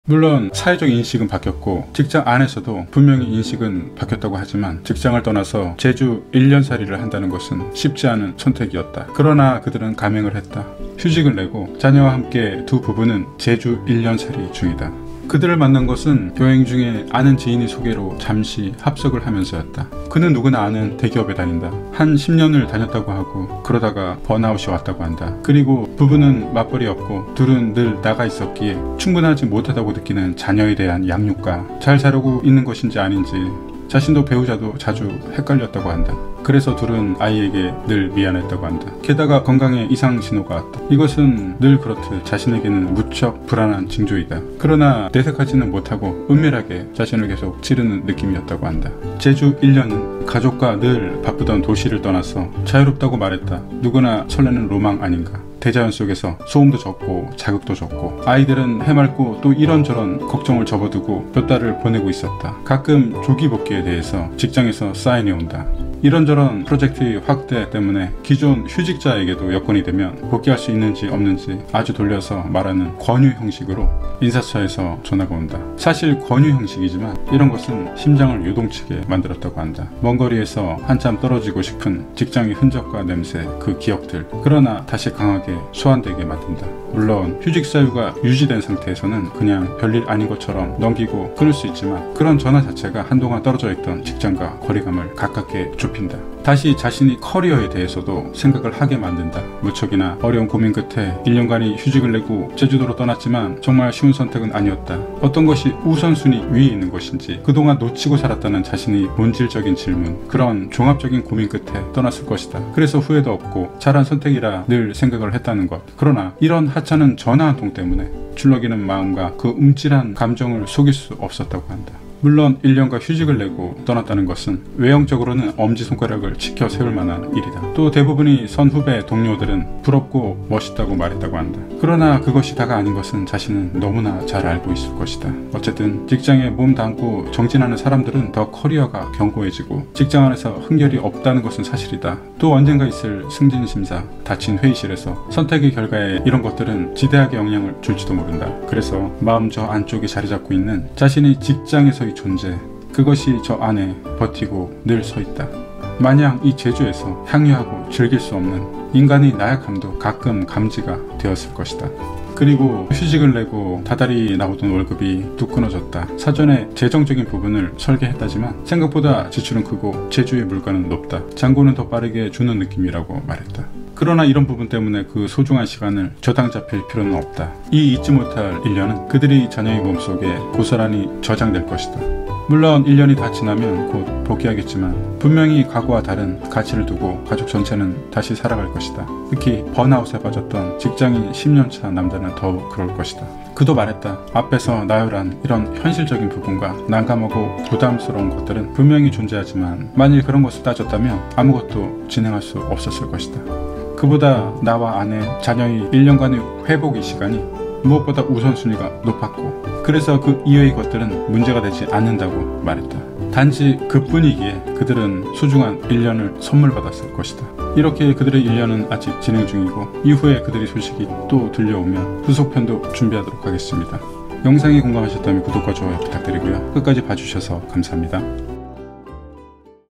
직장을 떠나 제주 1년살이하며 느낀것들(번아웃, 맞벌이, 부부동반 휴직)
제주도를 여행중 아는 지인의 소개로 만나게된 부부의 이야기입니다. 대기업을 다니다가, 부부 동반 휴직을 내고 아이와 함께 제주도에 1년간 정착 중이며, 제주 1년살이를 실행중입니다. 그들의 느낀점을 영상으로 만들었습니다.